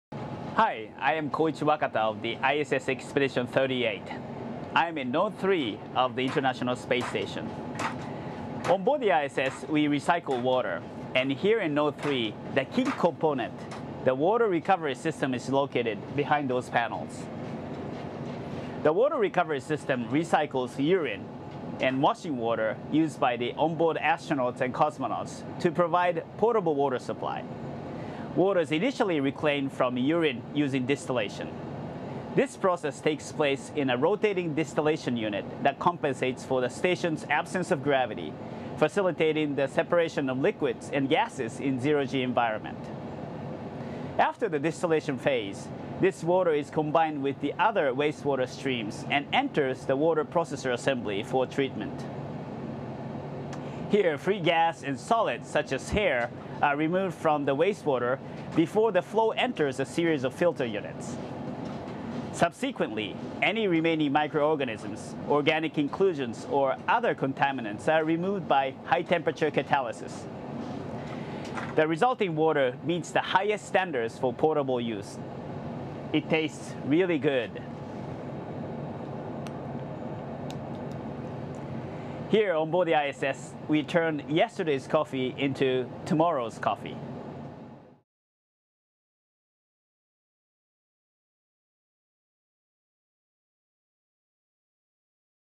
Recycling System Creates Clean Water on Space Station | NASA
- astronaut Koichi Wakata of the Japan Aerospace Exploration Agency discusses the the International Space Station's Water Recovery System, which recycles waste water into clean water.

Credit: NASA